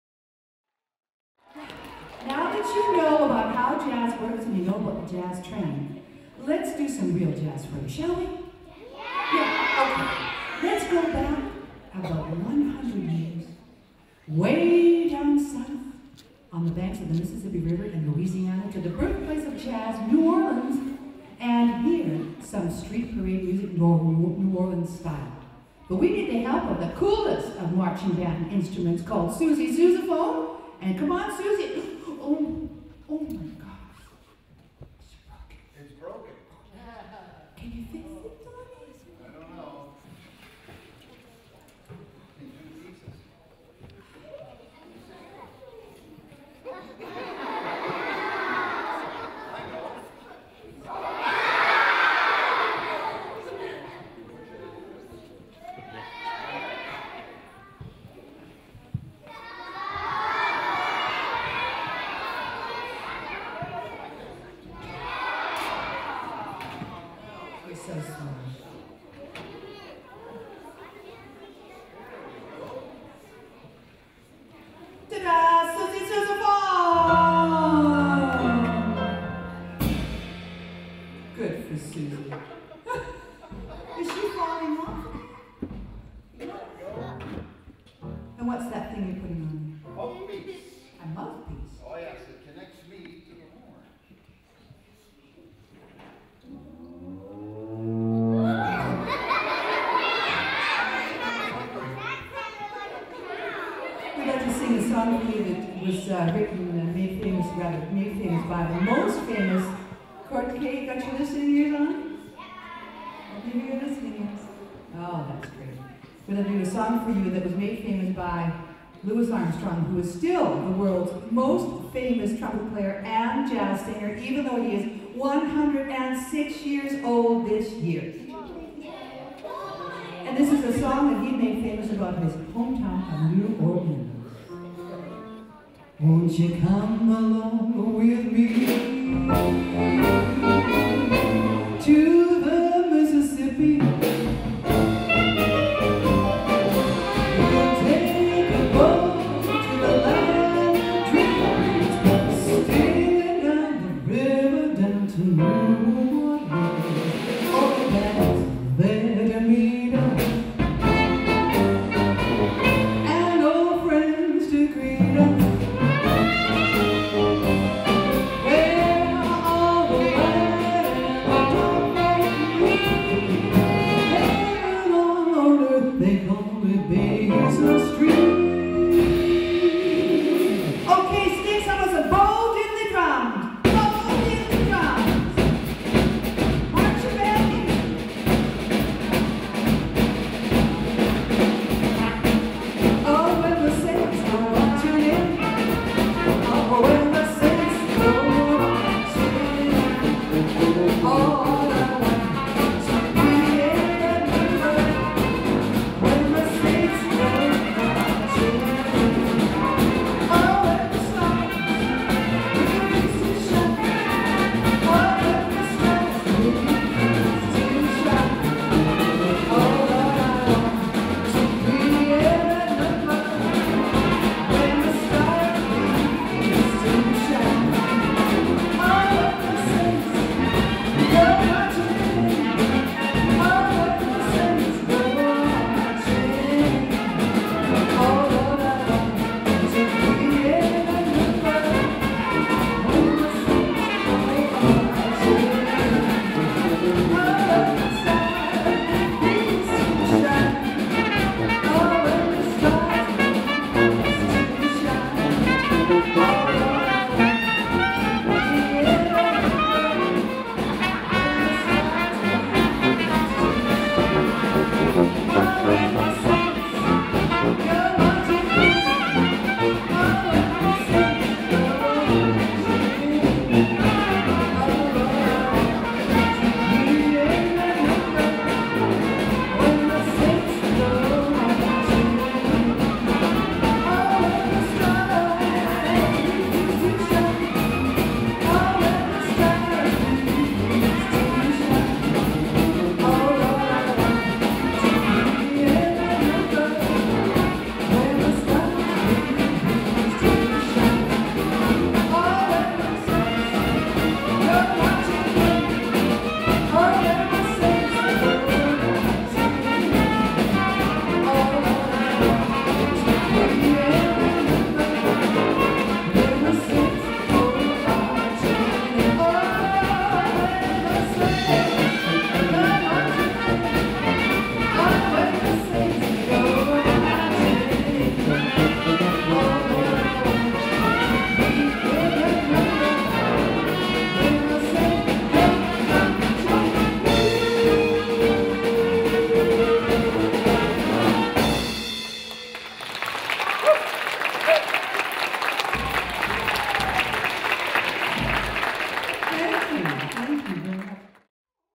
School of Cool with JazzAffair elementary school program
elementary school concert introduction to jazz